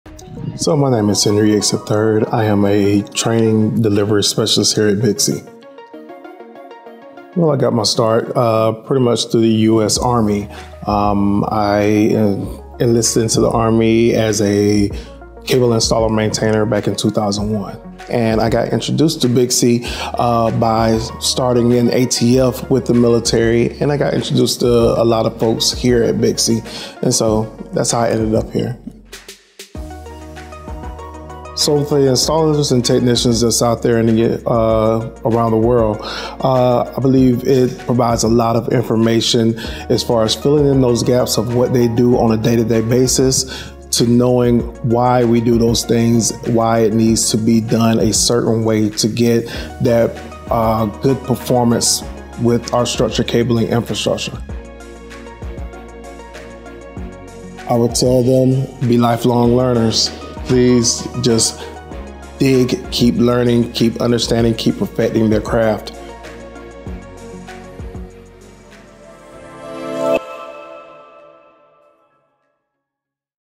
Interview with BICSI Training Delivery Specialist Henry Yates
BICSI Training Delivery Specialist Henry Yates III, TECH, INSTC, INSTF, CT, recently sat down with us for an illuminating conversation. In this video, Henry describes how his road to ICT started with his enlistment in the U.S. Army as well as discussing the benefits of taking BICSI courses for professionals in the field.